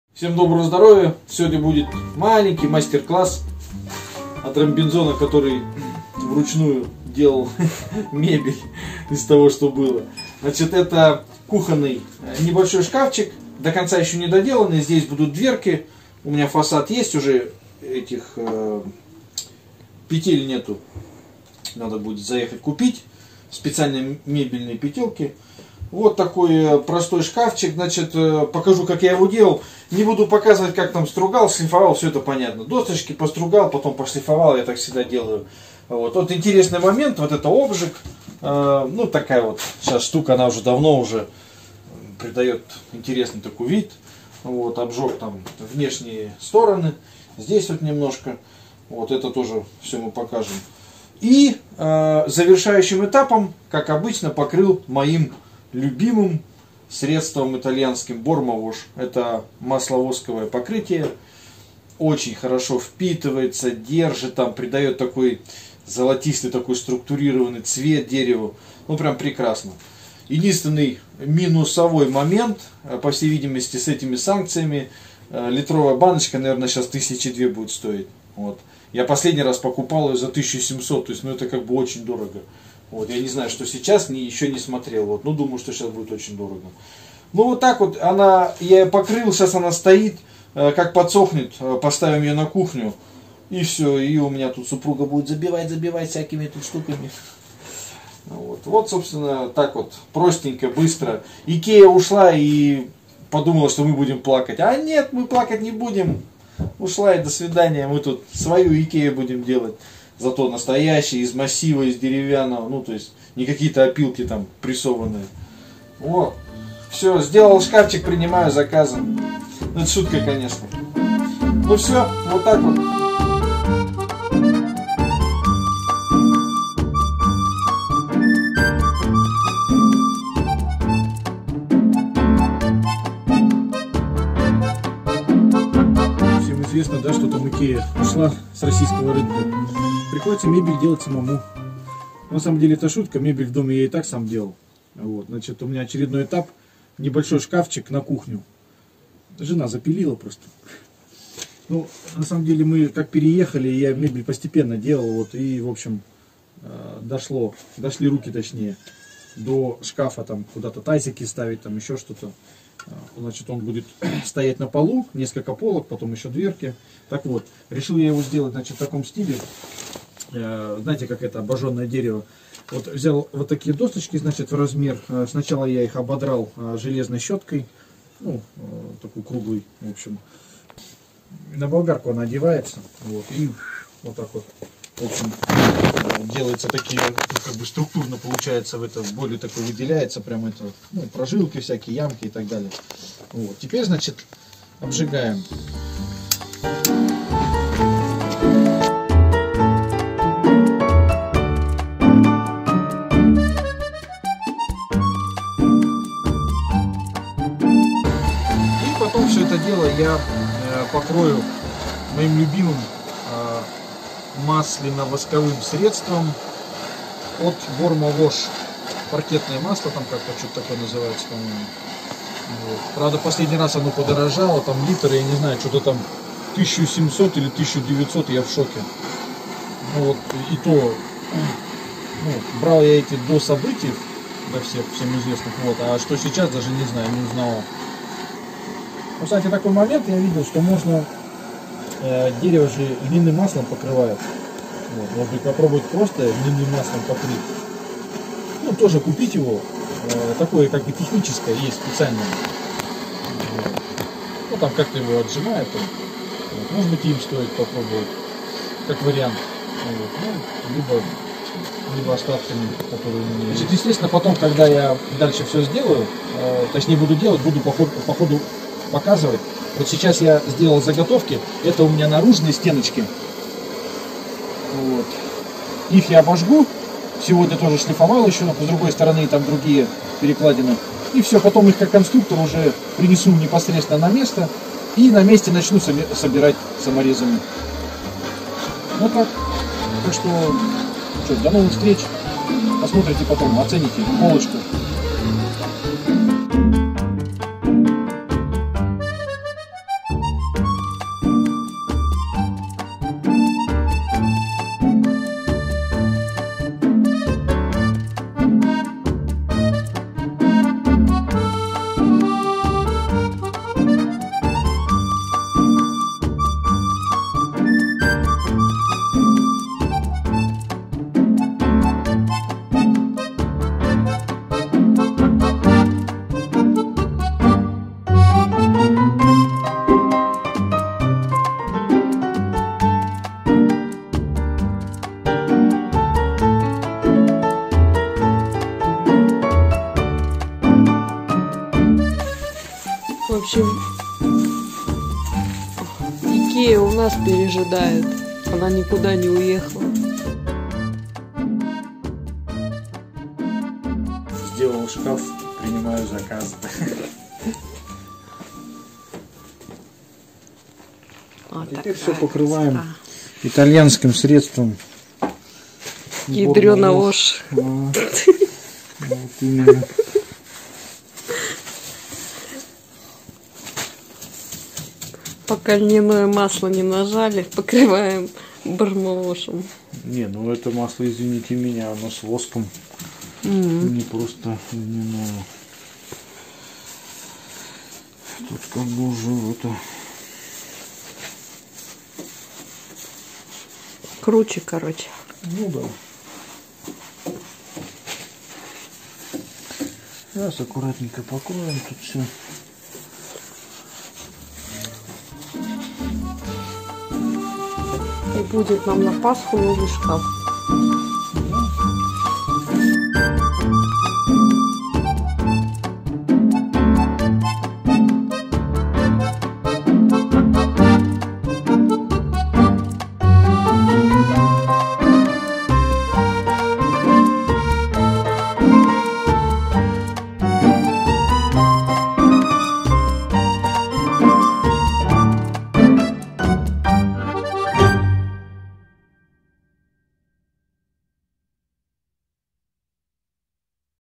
Сделал жене на кухню кухонный шкаф. Получилось дёшево, надёжно и намного лучше, чем IKEA. Мой обзор
Настоящему мужику несложно сделать самую простую мебель своими руками. Немного умения работы с инструментом и опыта. Самое главное не бояться. Делаю обзор своей поделки, даю немного полезных советов.